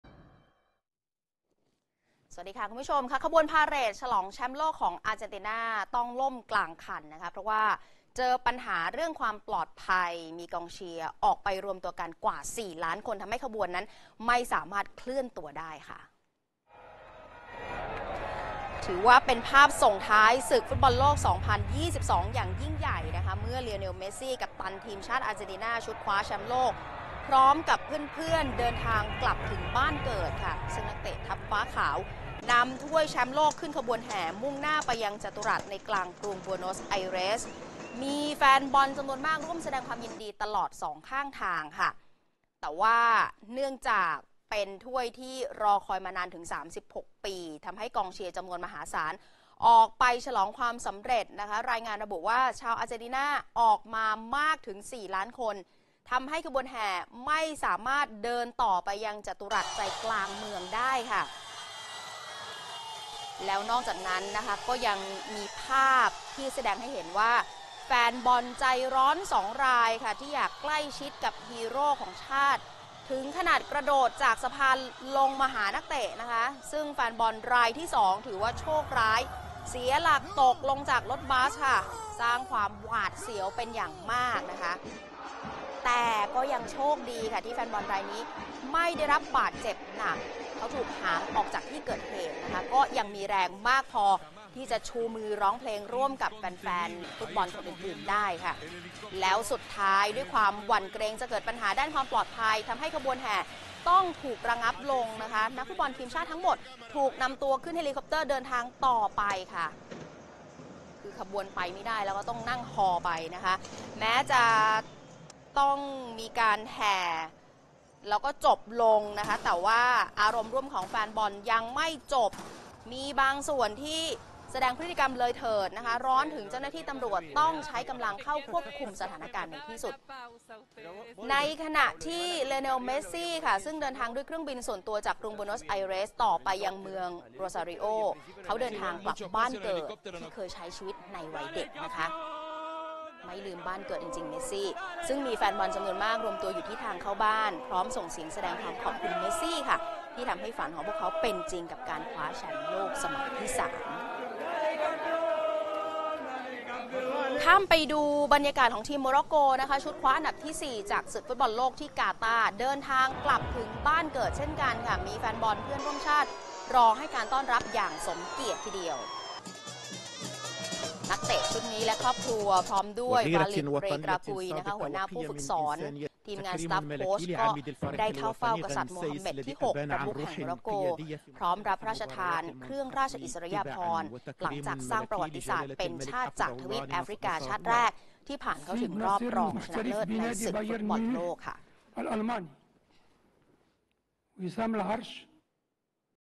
แฟนบอล 4 ล้านคน ทำพาเหรดฉลองแชมป์โลกล่ม | จับตาสถานการณ์ | 21 ธ.ค. 65
ขบวนพาเหรดฉลองแชมป์โลกของ อาร์เจนติน่า ต้องล่มกลางคัน เพราะปัญหาเรื่องความปลอดภัย หลังมีกองเชียร์กว่า 4 ล้านคนมาร่วมแสดงความยินดี